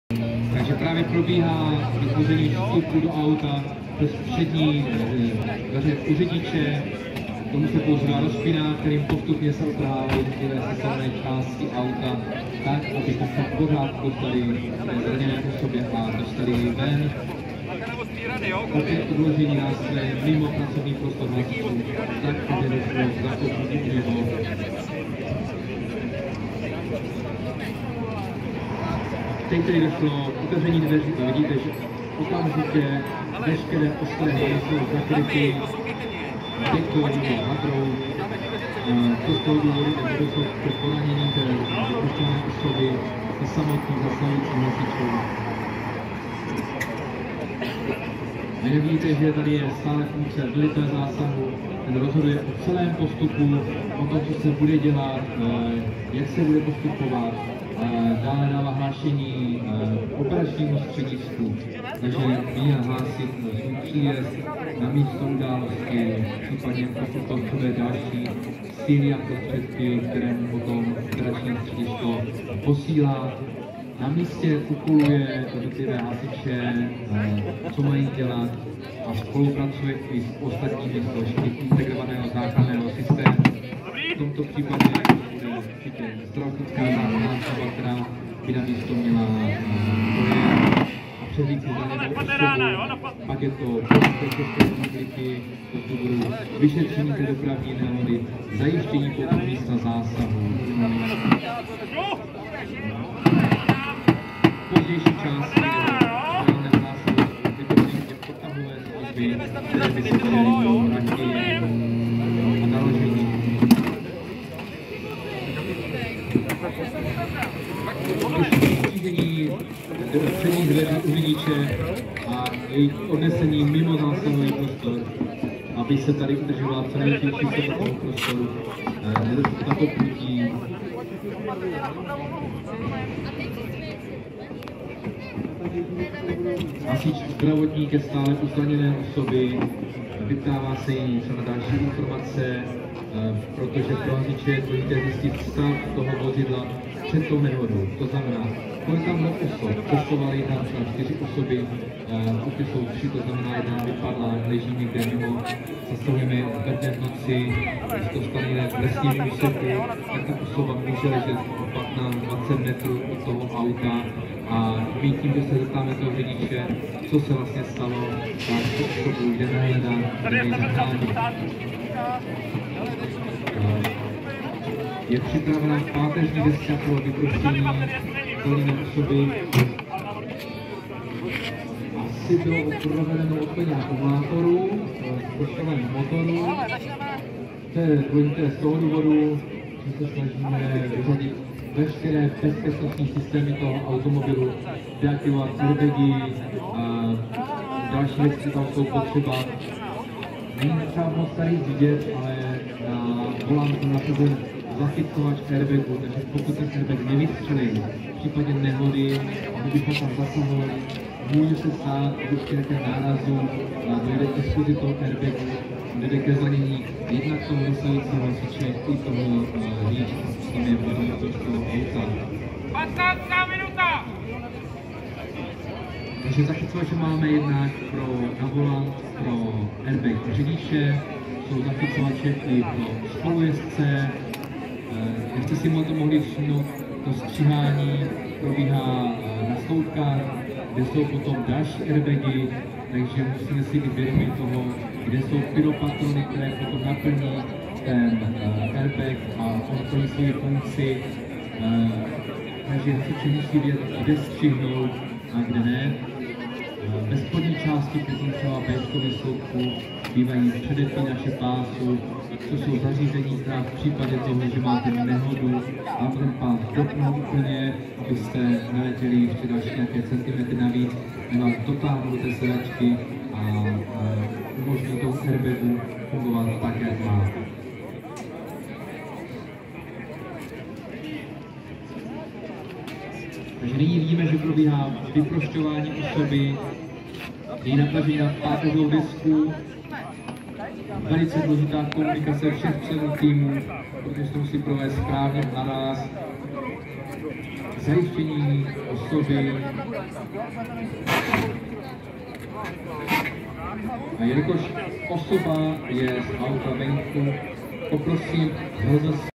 Vyprošťování u dopravních nehod Kroměříž - HZS Jihočeský kraj 2022
8.9.2022  - Mistrovství republiky hasičů ve vyprošťování u dopravních nehod Kroměříž  - HZS Jihočeský kraj  - Územní odbor Jindřichův Hradec